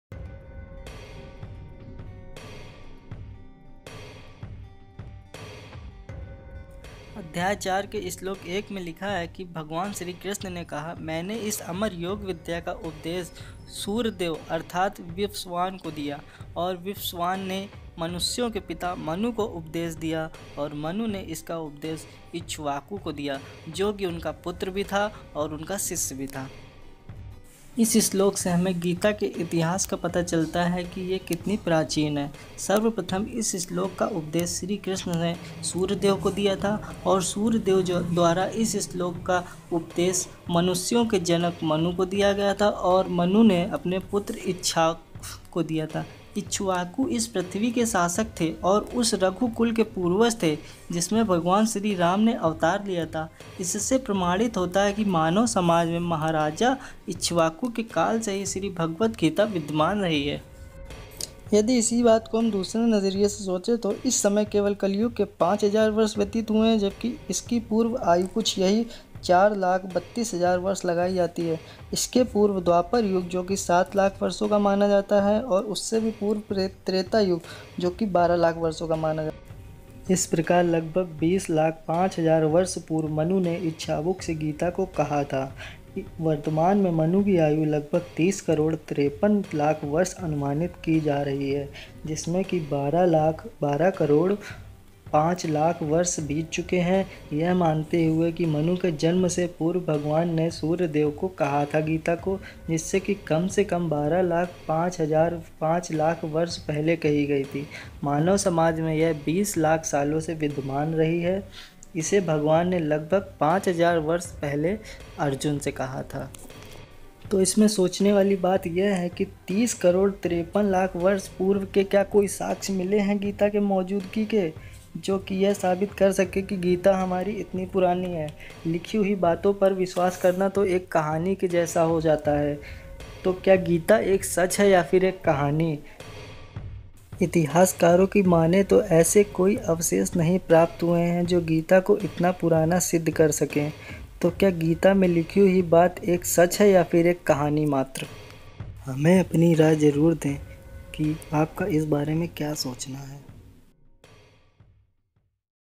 धर्माअध्याय ( भाग-1 ): कितना पुराना है गीता का प्रथम उपदेश, सर्वप्रथम किसको दिया था उपदेश..
30 करोड़ 53 लाख साल पुरानी है गीता, 20 लाख सालों से है मानव समाज में, और इस प्रकार के कई राज है इस वीडियो में।